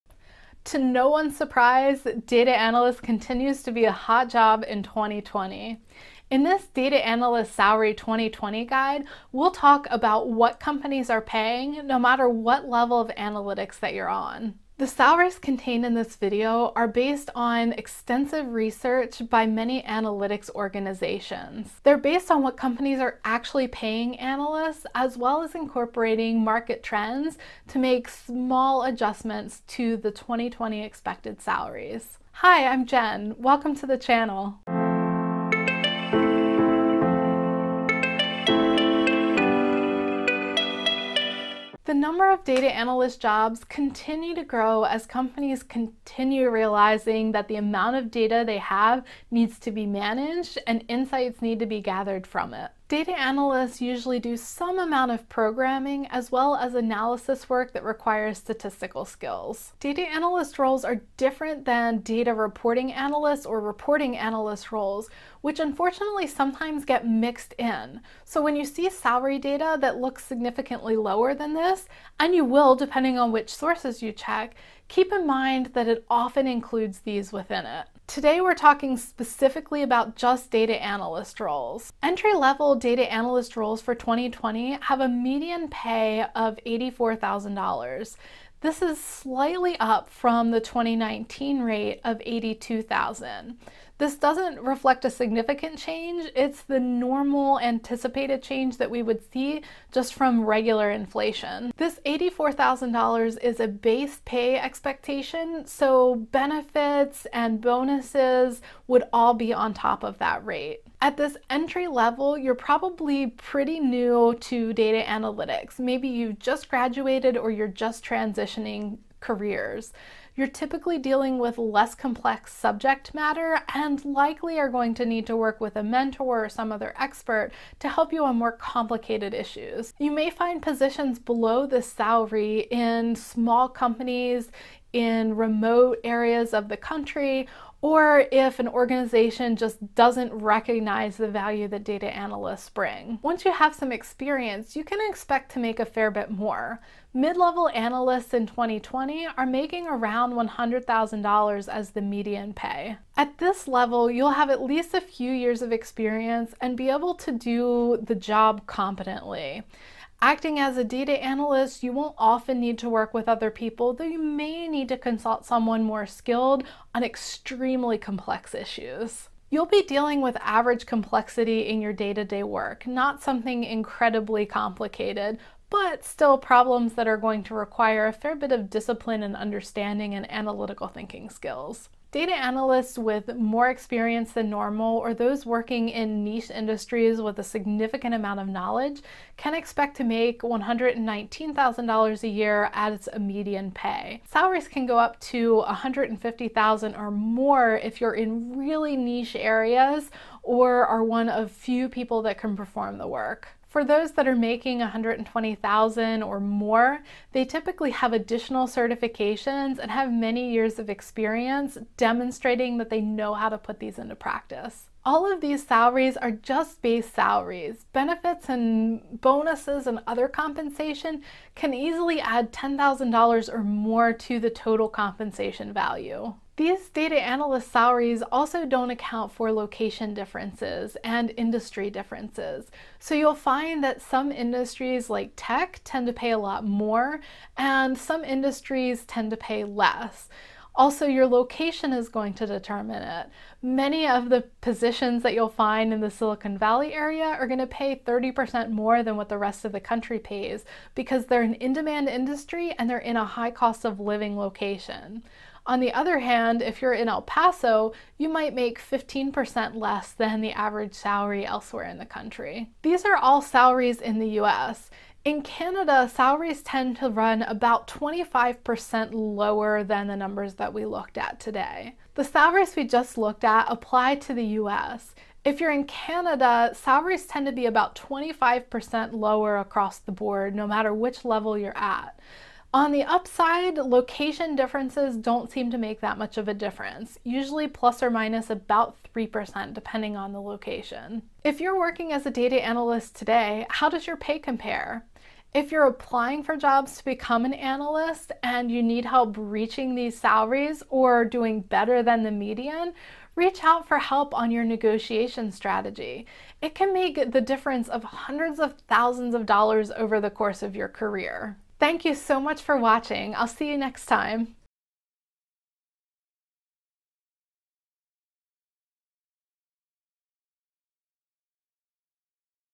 Data Analyst Salary 2020
DATA ANALYST SALARY 2020 // Find out what you can expect as a data analyst salary in 2020 based on hiring data and predictions.

Data analytics careers pay quite well. With starting salaries in the upper 5 figures and easily crossing into 6 figures with some experience, data analyst pay ranges substantially based on your experience.

Data analyst continues to be a growing position and offer good room for career and salary growth.

This video is a specific guide to US data analyst salary and Canada data analyst salary.